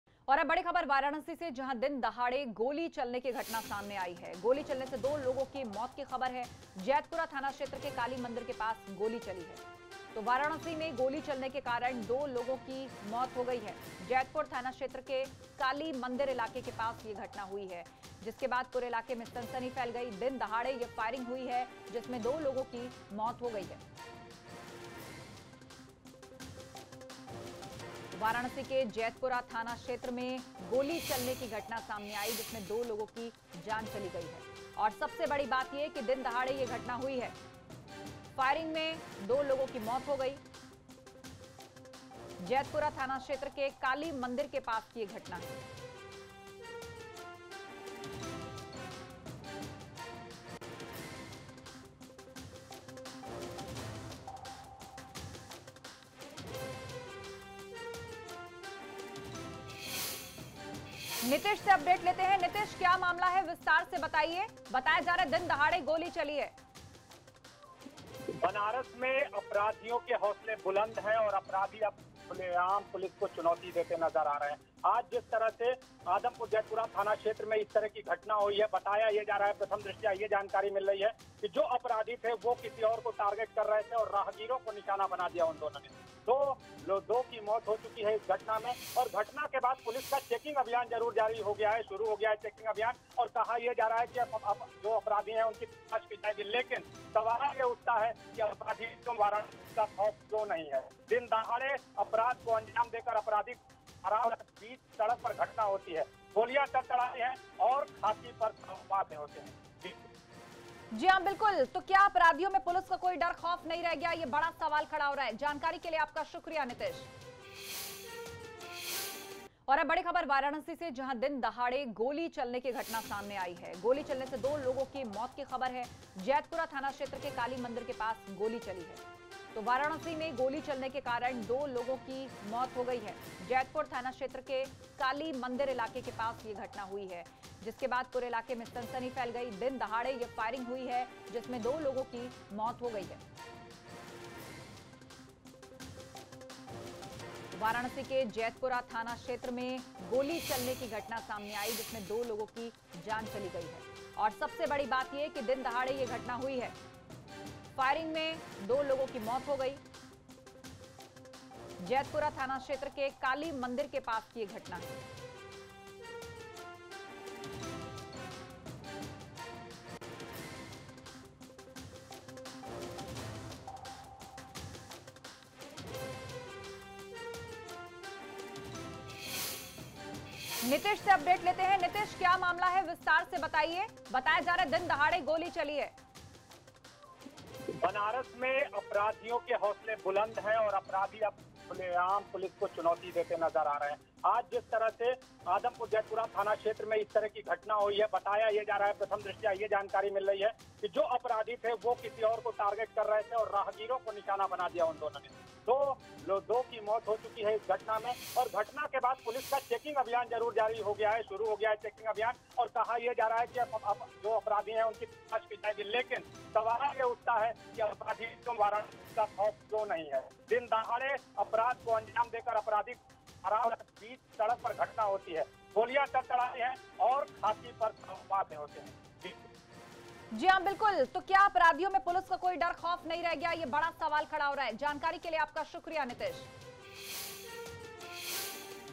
दिनदहाड़े फायरिंग में गई दो लोगों की जान। Varanasi Firing | ABP Ganga
वाराणसी में दिनदहाड़े गोली चलने से हड़कंप मच गया है. फायरिंग में दो लोगों की मौत हो गई है. ये घटना जैतपुरा थानाक्षेत्र के काली मंदिर के पास की है. फिलहाल पुलिस मामले की जांच कर रही है.